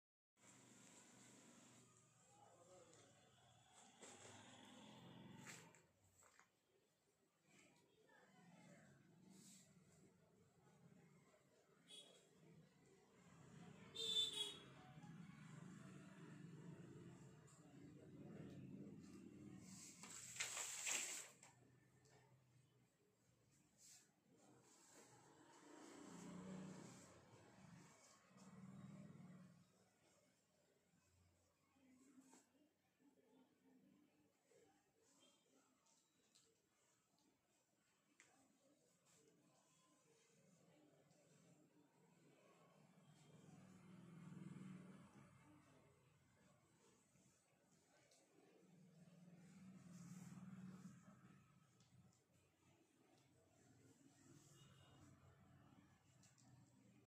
ILAMPILLAI *_Trendy & Fancy Soft Silk Sarees_* Lite weight
*_Trendy & Fancy Soft Silk Sarees_*

Lite weight sarees with grand  work in Pallu looks pretty..

Soft texture with feel like cool...

_Contrast Plain Running Blouse_

_Pretty high Attractive Silver Bhutta All-Over Body_